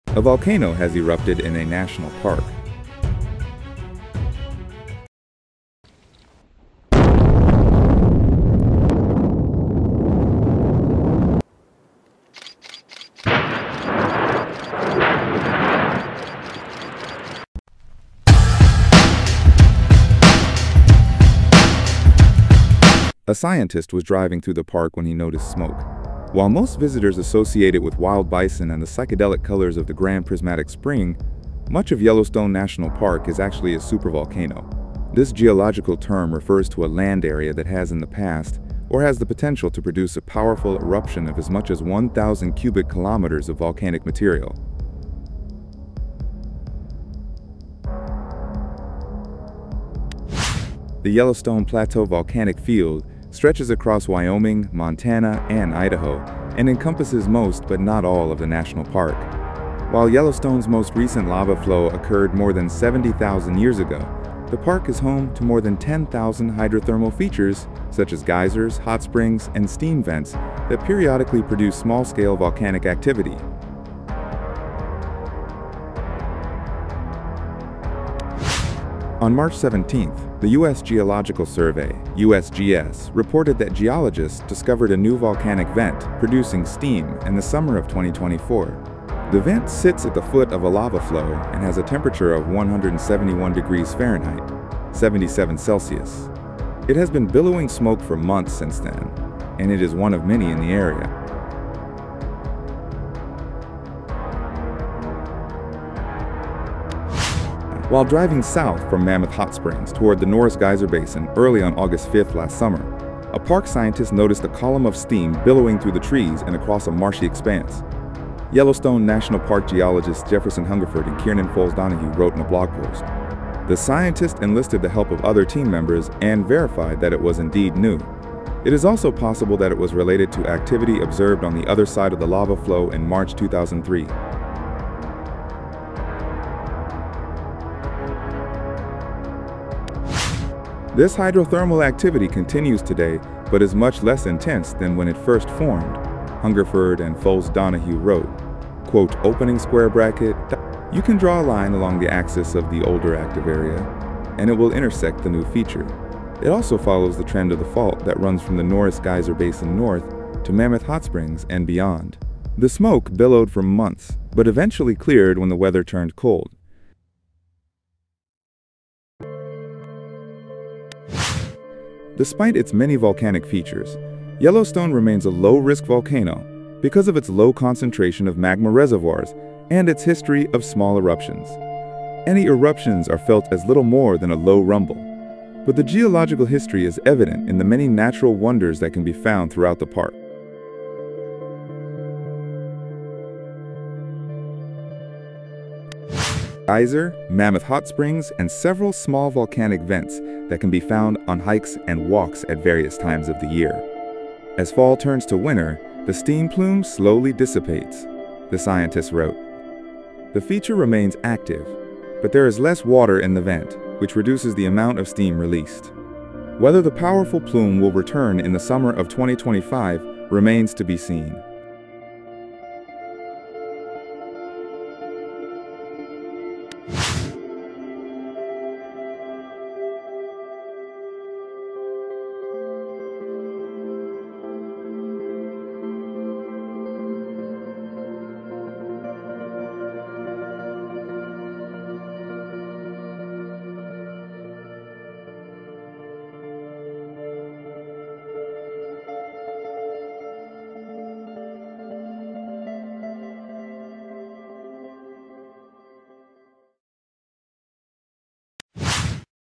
US Panic: 1 hour before Yellowstone volcano erupts, as earth rises, rumbling across land
A scientist was driving through the park when he noticed smoke. While most visitors associate it with wild bison and the psychedelic colors of the Grand Prismatic Spring, much of Yellowstone National Park is actually a supervolcano. This geological term refers to a land area that has in the past or has the potential to produce a powerful eruption of as much as 1,000 cubic kilometers of volcanic material.

Yellowstone supervolcano:volcanoeruption